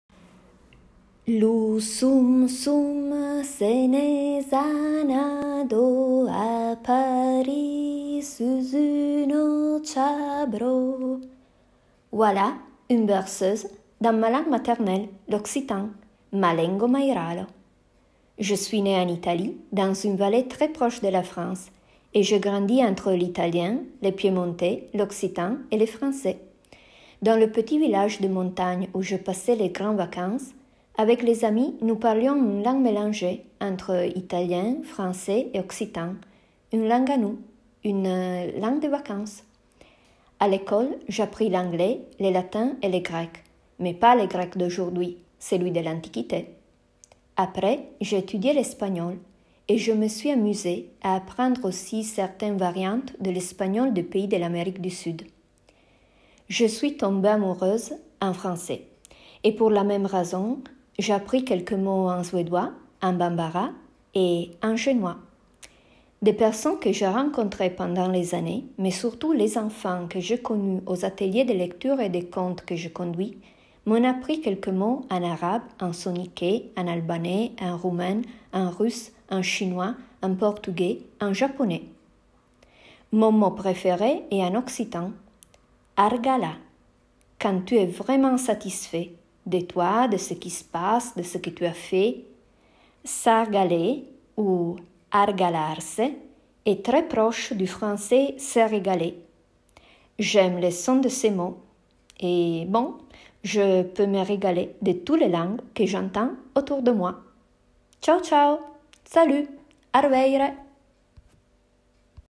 Biographie langagière sonore pour le calendrier de l'avant-les-vacances 2020
Dans le cadre de son calendrier de l'avant-les-vacances 2020, Dulala vous fait découvrir quotidiennement des biographies langagières du 1er au 18 décembre. Vous aussi, envoyez-nous votre biographie langagière pour nous raconter les langues qui vous entourent !